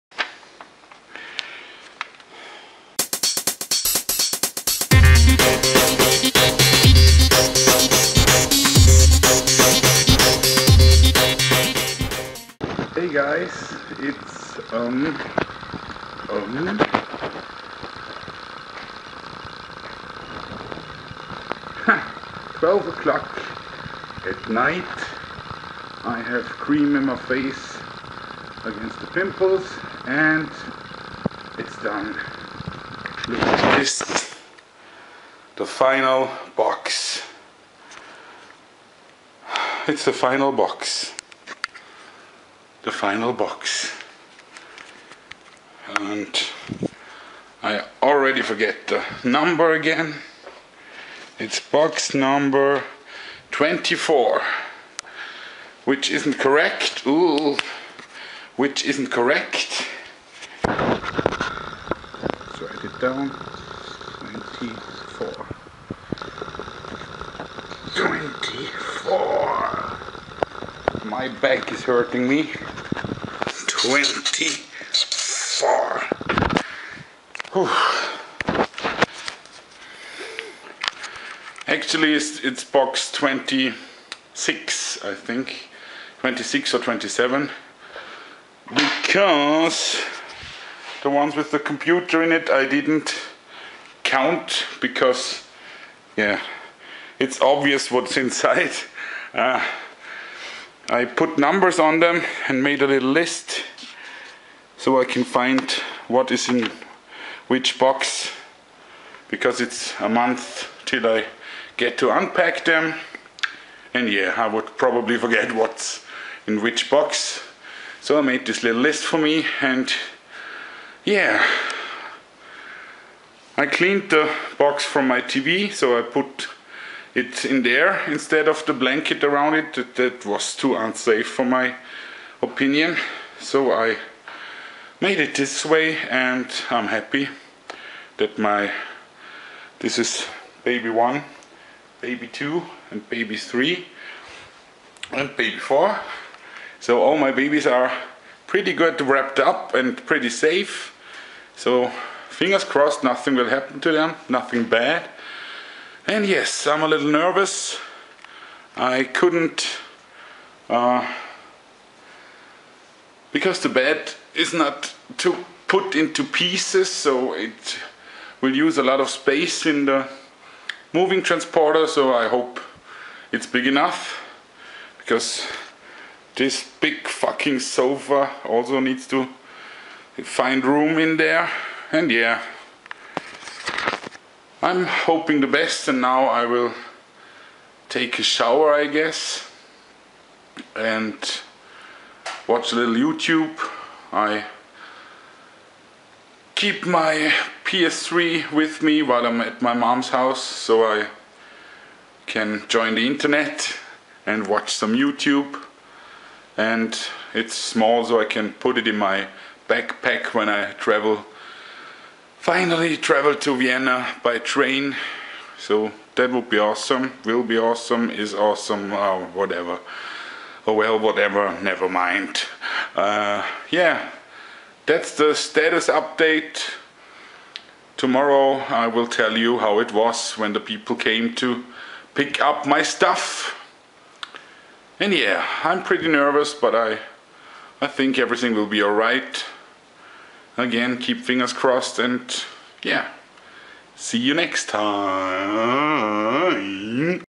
Moving to Vienna - Der Xiberger Vlog 6 - the real end of the last day of packing :)
Packing is over! yay!